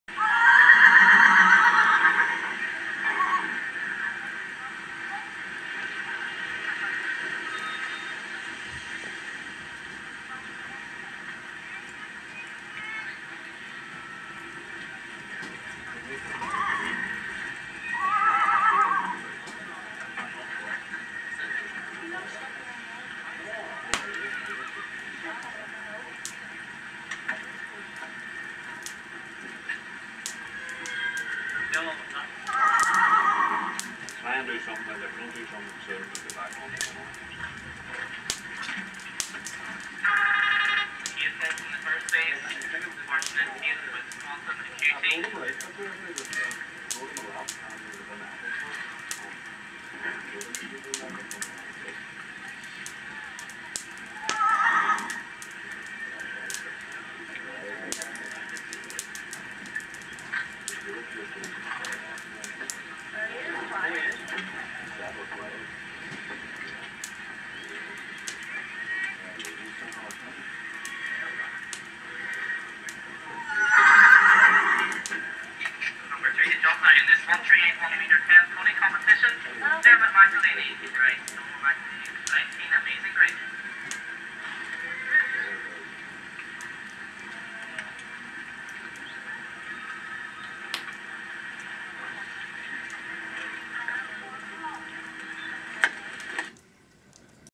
joey
joeys 1st grand prix limavady
8 faults but not bad for a cart horse!